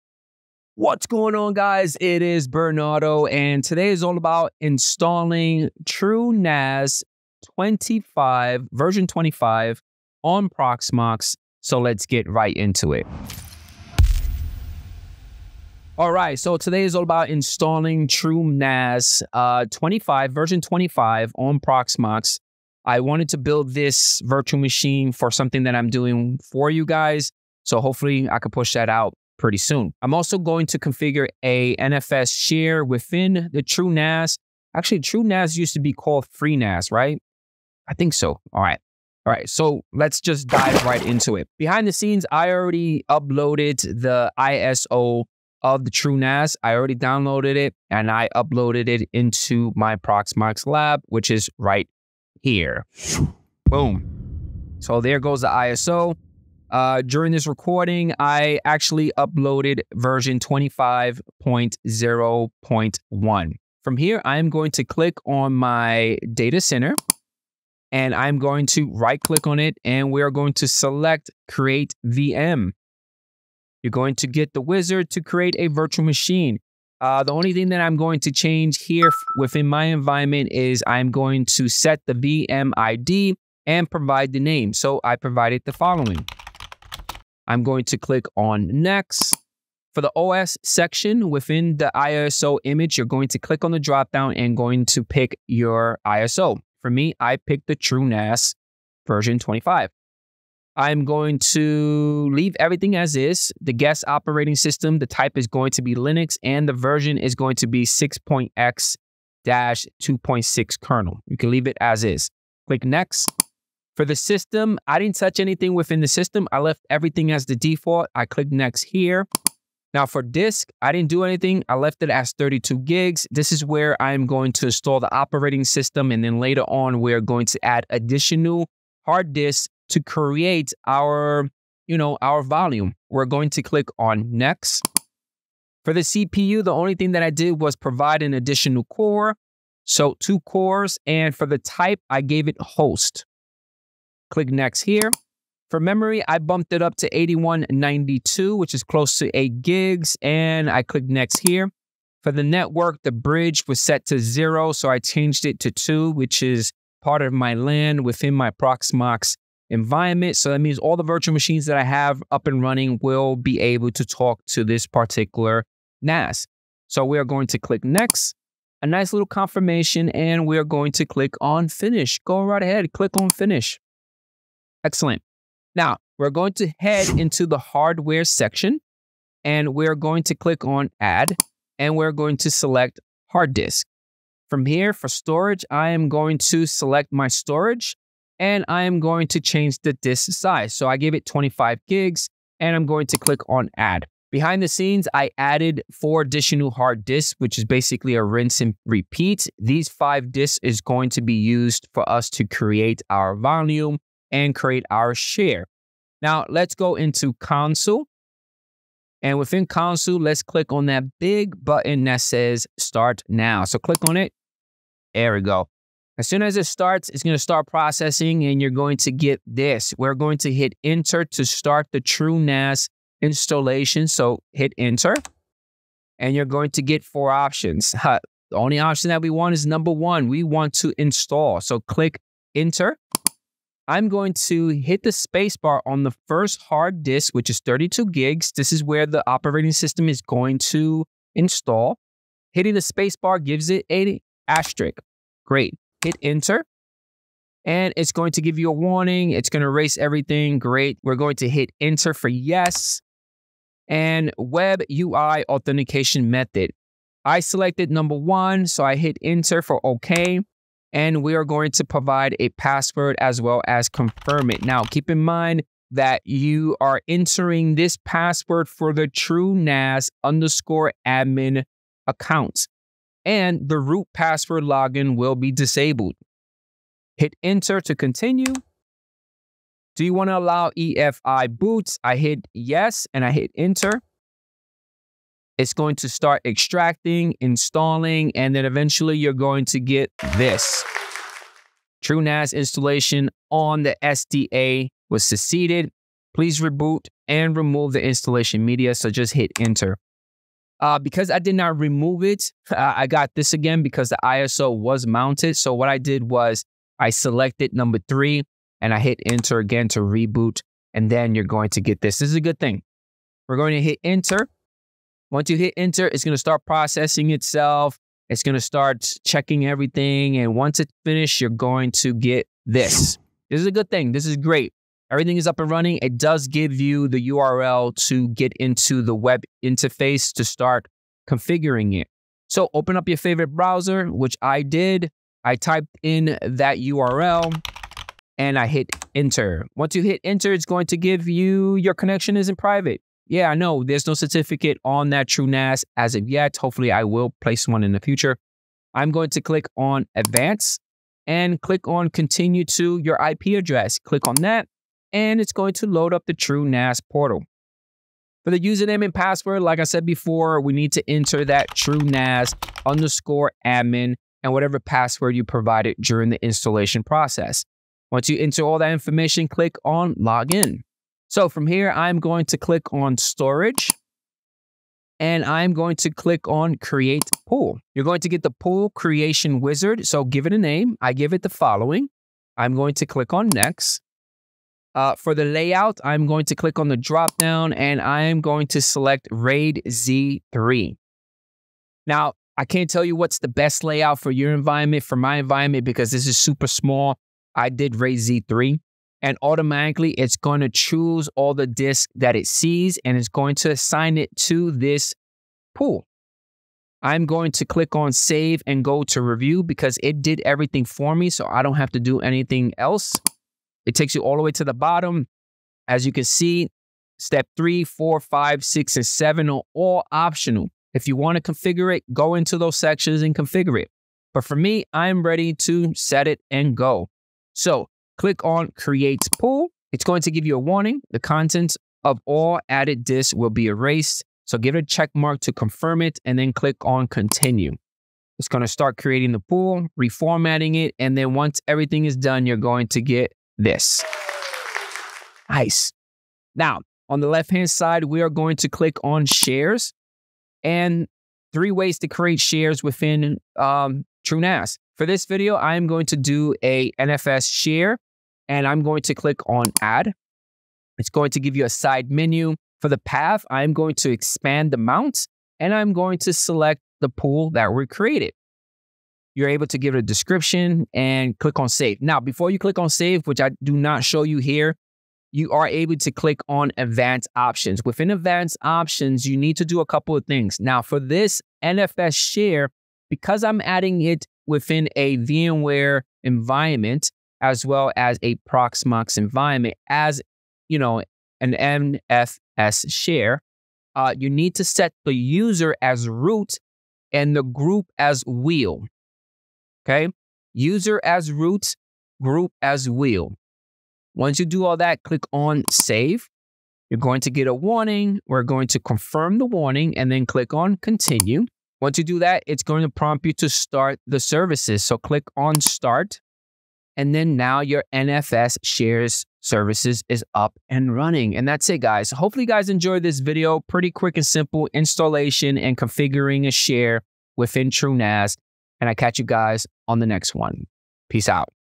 Install & Configure TrueNAS 25 on Proxmox VE | Step-by-Step Setup Guide
Learn how to install and configure TrueNAS 25 on Proxmox Virtual Environment (VE) in this step-by-step guide.

Chapters
0:00 Introduction
0:13 Logo Introduction
0:15 Video overview
0:52 TrueNAS ISO Image
1:07 Create Proxmox VM for TrueNAS
2:58 Add additional disk in VM
3:31 Start TrueNAS VM
3:41 Install TrueNAS
5:35 Log into TrueNAS portal
6:13 Create TrueNAS storage pool
7:28 Create NFS Share
8:41 Conclusion